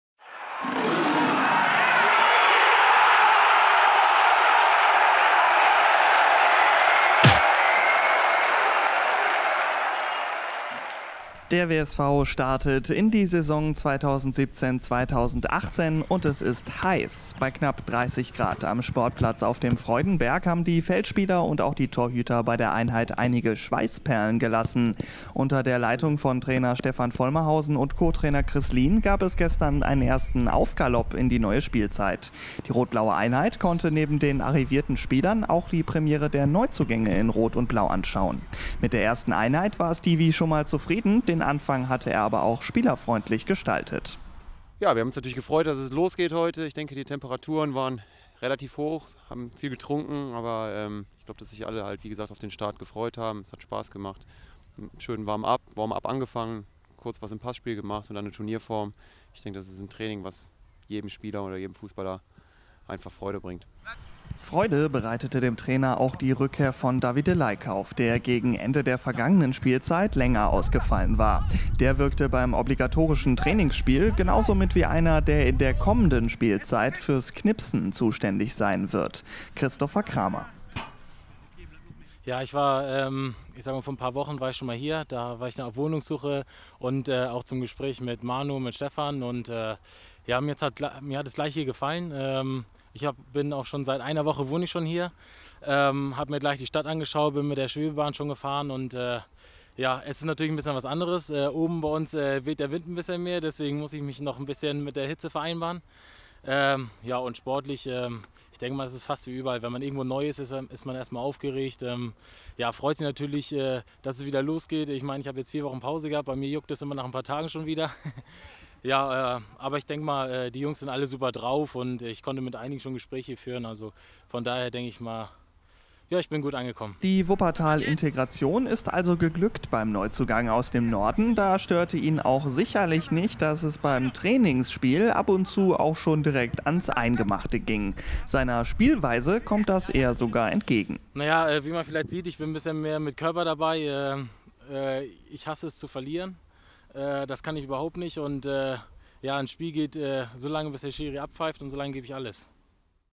WSV-TV: Trainingsauftakt Saison 17/18
Da kamen alle Spieler des WSV gut ins Schwitzen: Der Trainingsauftakt auf dem Freudenberg bei 30 Grad gestern gibt den Auftakt in die neue Spielzeit! Wir haben mit Cheftrainer Stefan Vollmerhausen und unserem Neuzugang Christopher Kramer über ihre ersten Eindrücke in der Saison gesprochen!

Viel Spaß!